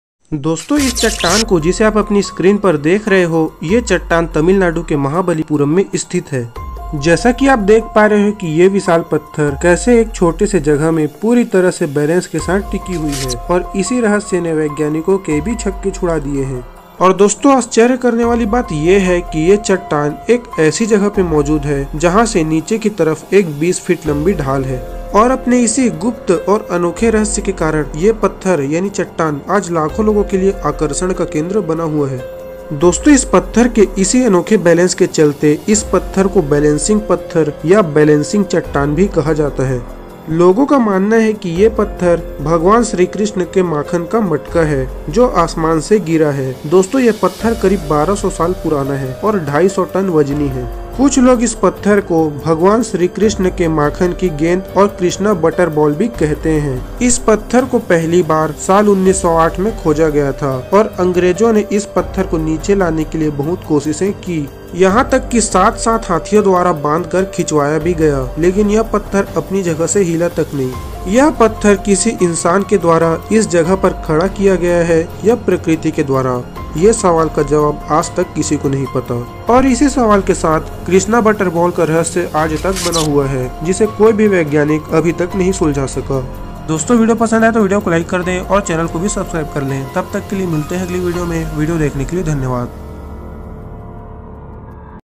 एक ऐसा पत्थर जिसने वैज्ञानिकों के भी होश उड़ा दिए || Balancing Stone Analysis
In this video you will know the complete information of Balancing Stone in very short time and in very easy way. What is a balancing stone and where is it located and what is its speciality Full Details. What is krishna butterball.
What is mahabalipuram special Stone.

This is some important point in this video-

a. What is balancing stone
b. Where is the balancing stone located
c. What is the speciality of balancing stone
d. what is krishna butterball
e. why krishna butterball does not move
f. balancing Stone , mahabalipuram krishna butterball analysis and full details.

balancing Stone mahabalipuram Tamilnadu
giant balancing rock
giant balancing Stone on a slope